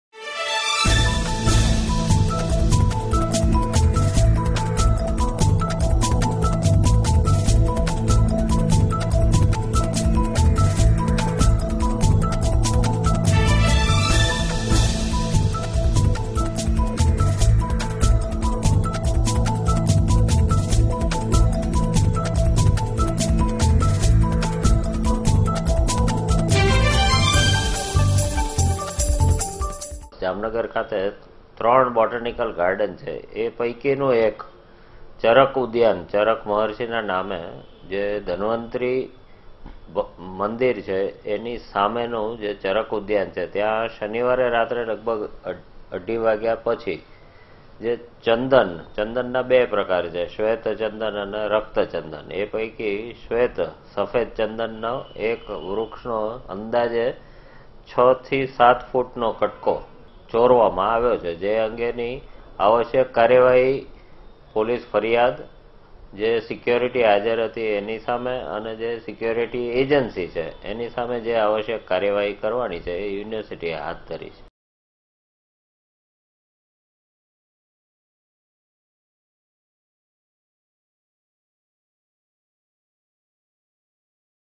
Jamnagar ના ગુજરાત યુનિ. માં સફેદ ચંદનના વૃક્ષની ચોરી | WhiteChandan | GTPL
Jamnagar ના ગુજરાત યુનિ. માં સફેદ ચંદનના વૃક્ષની ચોરી | WhiteChandan | jamnagar | Nirmana News | GTPL